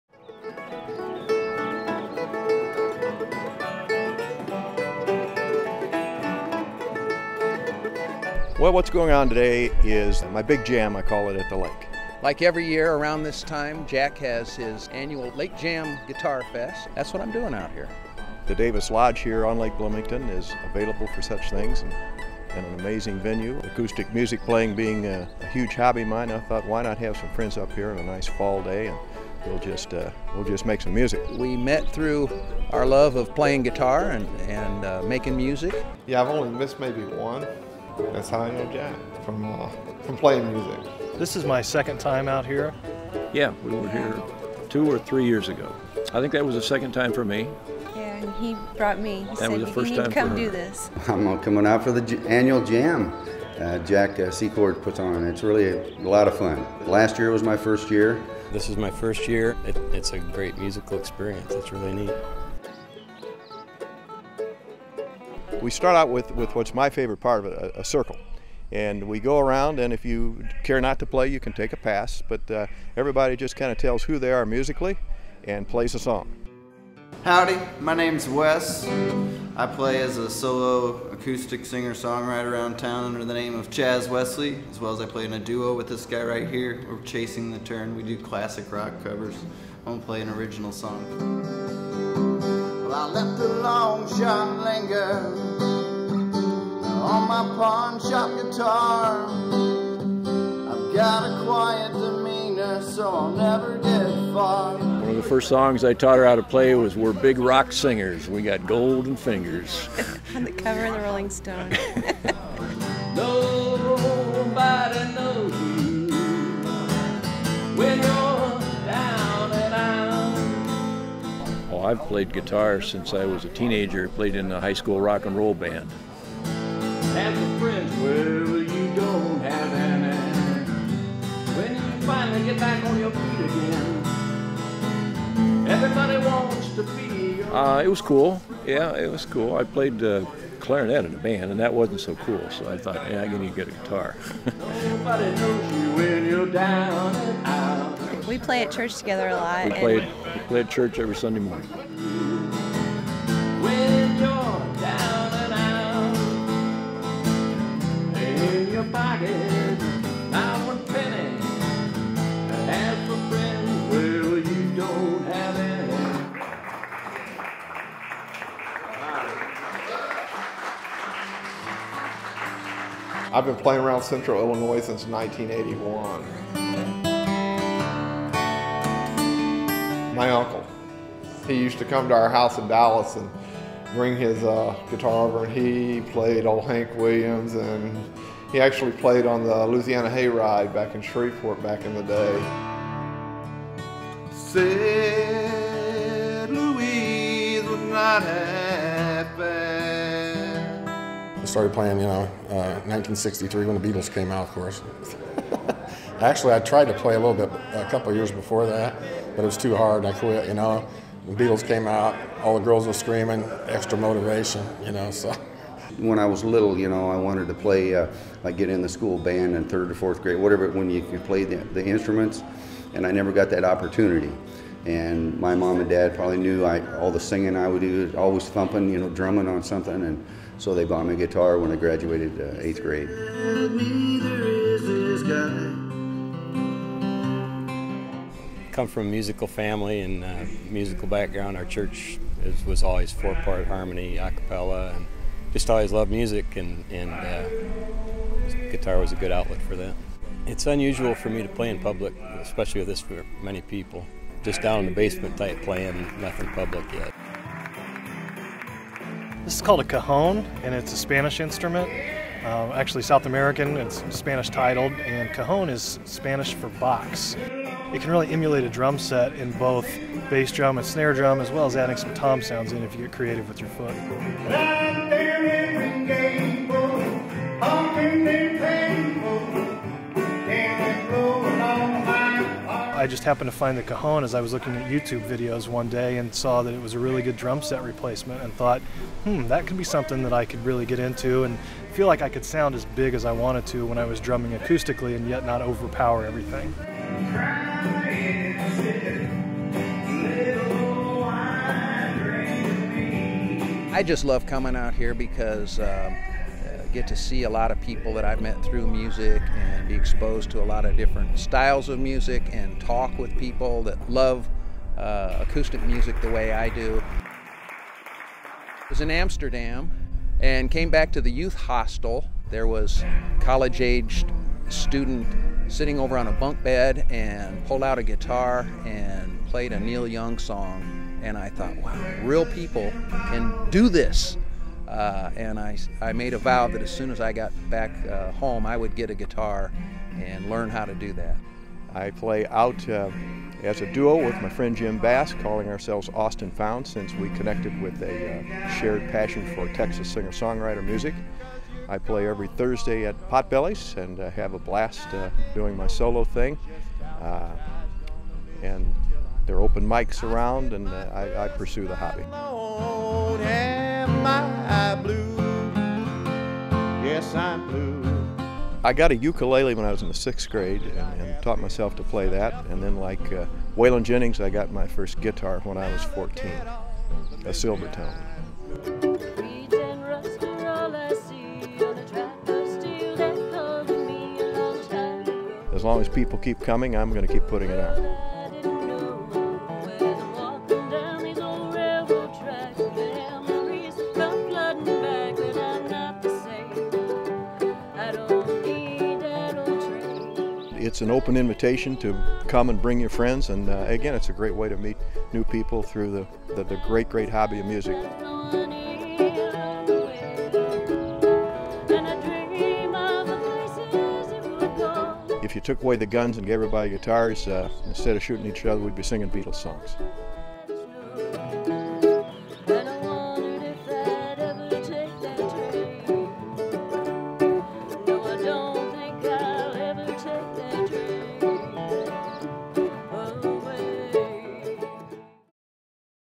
LakeJamDoc.mov
Each fall financial planner Jack Secord of Bloomington, IL hosts his acoustic music lake jam at Davis Lodge on Lake Bloomington. This mini documentary takes a look at the 2011 Jam held on Oct. 8.

Photographed, directed, and edited by Brent Simonds (guitarist with Cross the Dog).